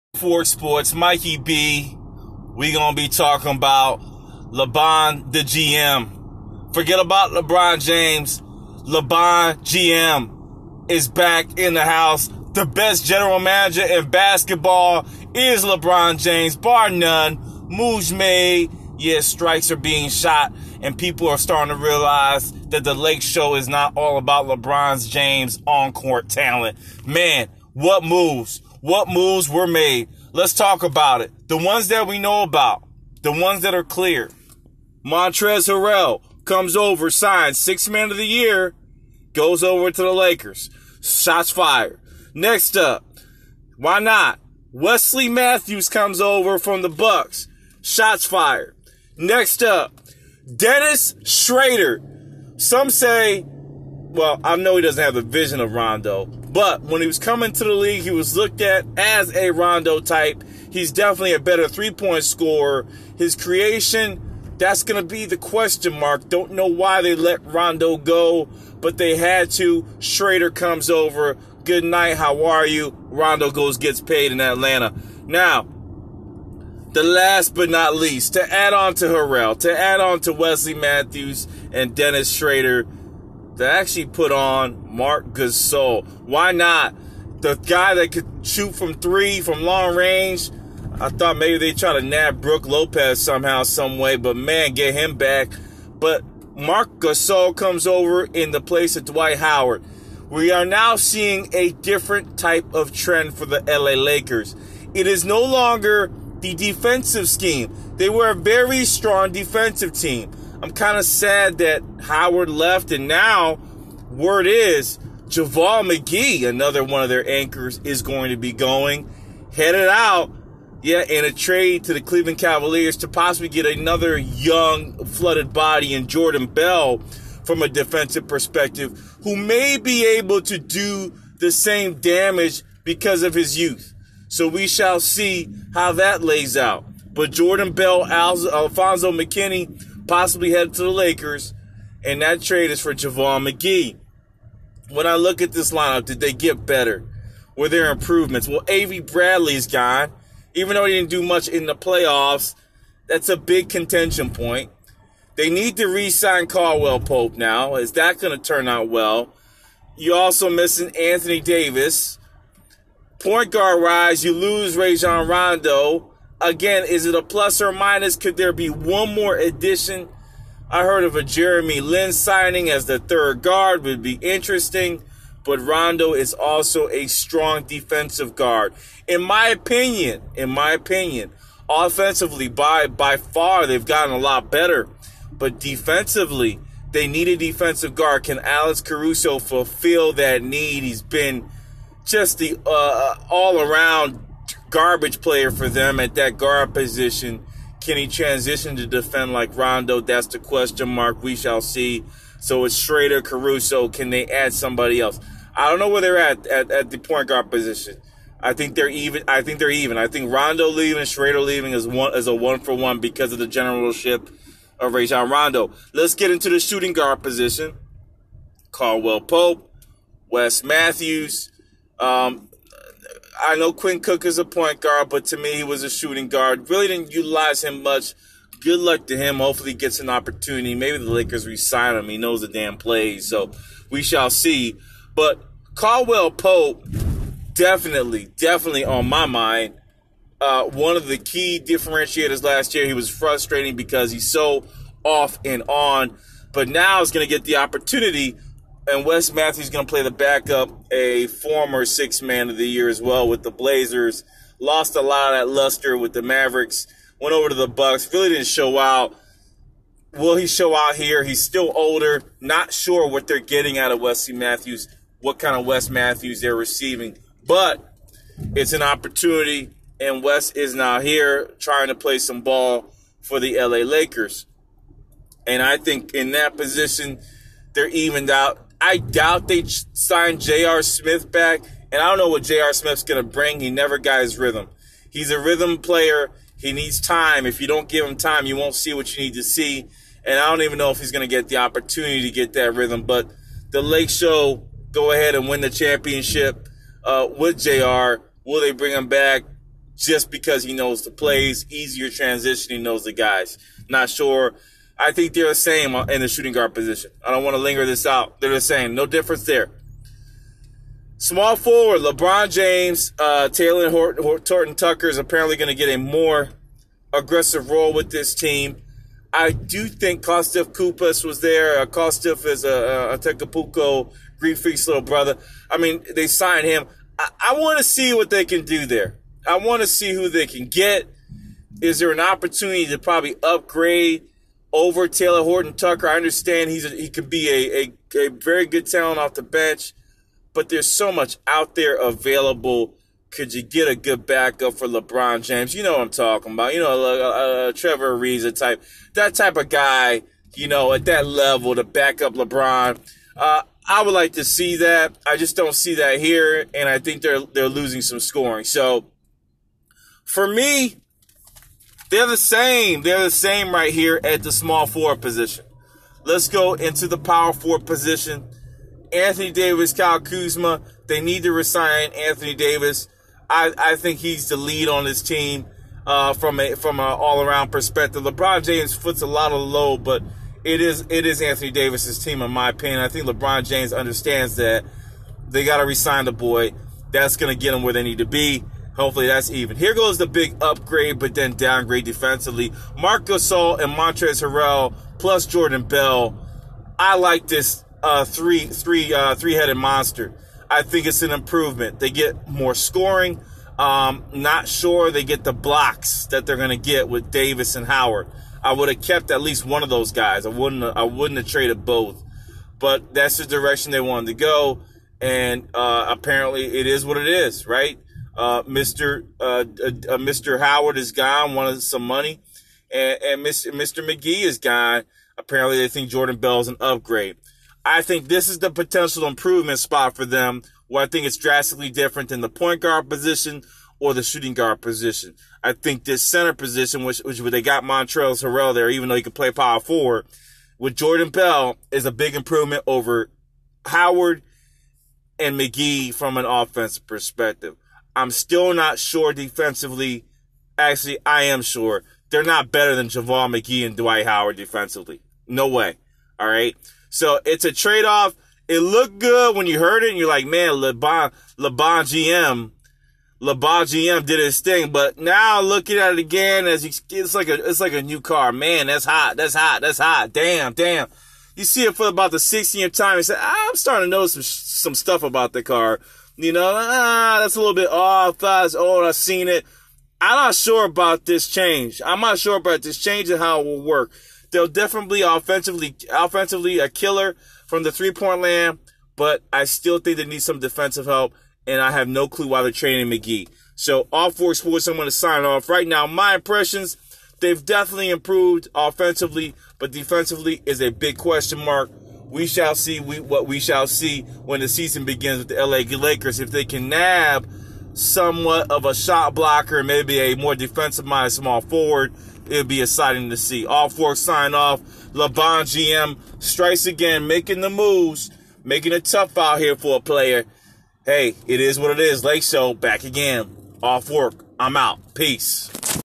Lakers new roster from LeGM moves
it's all about the roster changes for LeBron James making his team a little bit more special with the addition of Marc gasol montrezl Harrell there Schrader and Wesley Matthews